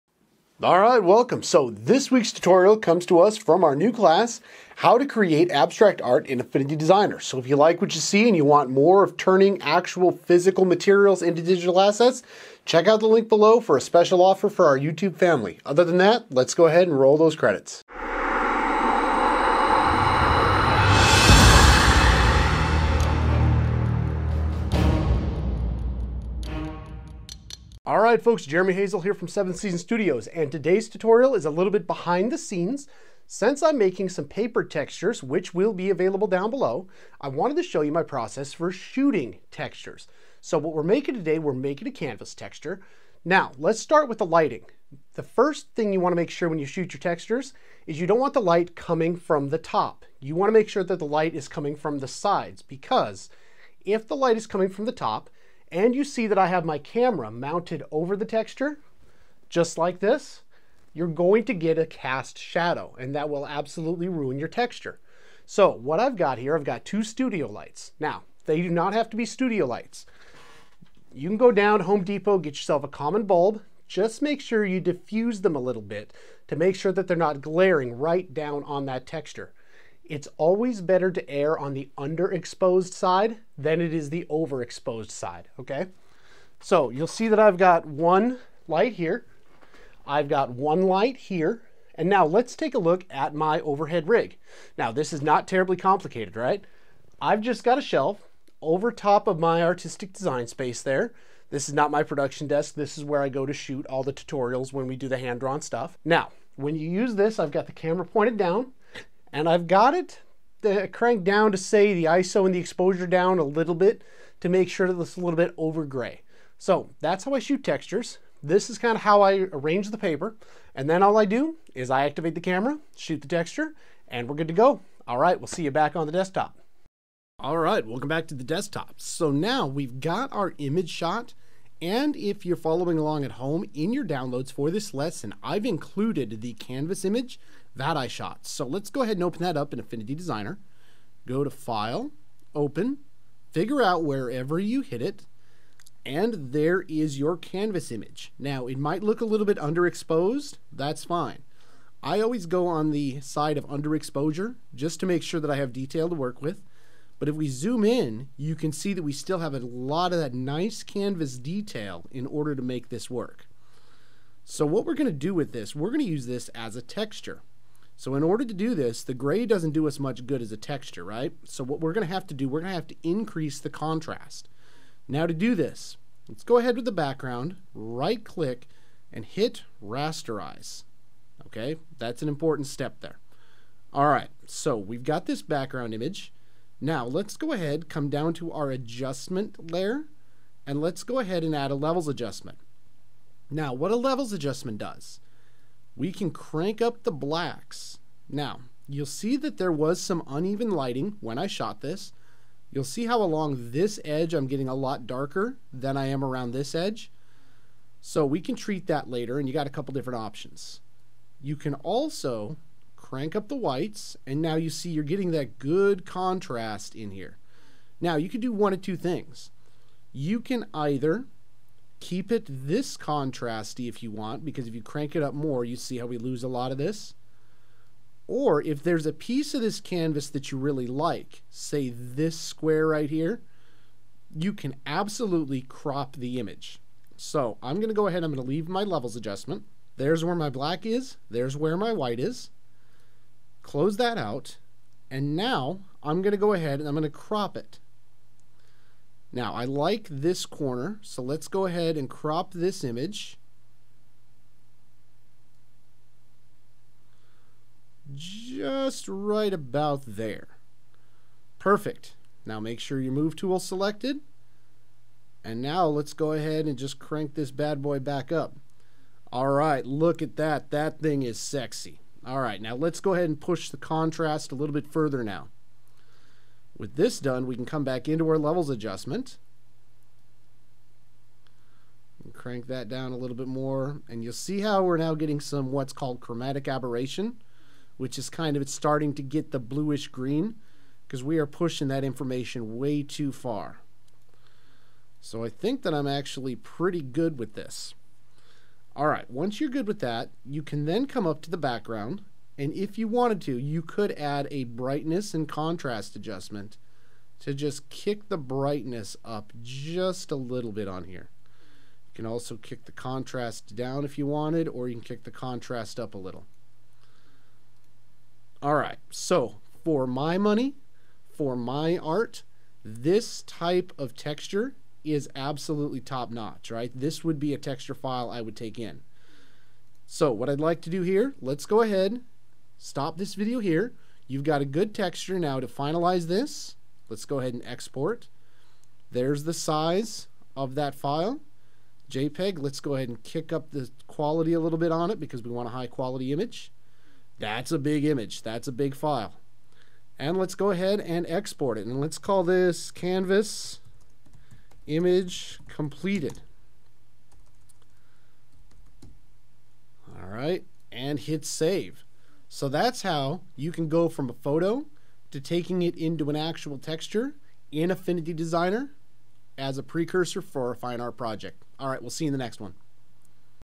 Making a canvas texture in Affinity Designer and Affinity Photo
In this video we show you how I shoot and create digital canvas textures and prepare them for assets in Affinity Photo and Affinity Designer. Canvas textures have substantial impact on your art and can really give that hand drawn feel when working in digital.